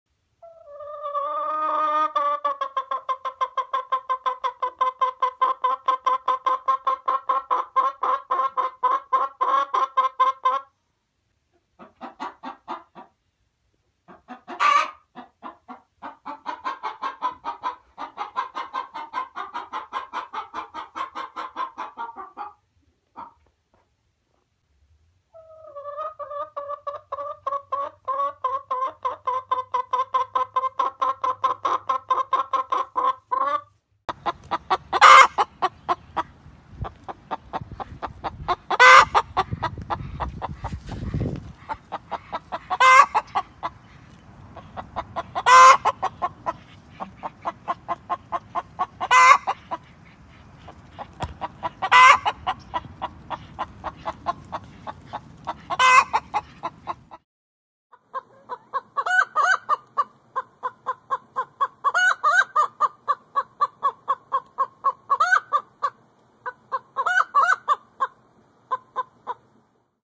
Chicken sounds, hen clucking
If you like animals and sounds of nature, this video is for you! In this video you will hear what sounds chicken makes, how chicken talks, and you will see very beautiful and funny pictures with chickens and their babies.

Enjoy this video, happy watching!

Chicken
鸡
मुर्गी
Gallina
Gallus gallus domesticus
Курица
دجاج
মুরগি
Galinha
Ayam
Haushuhn
ニワトリ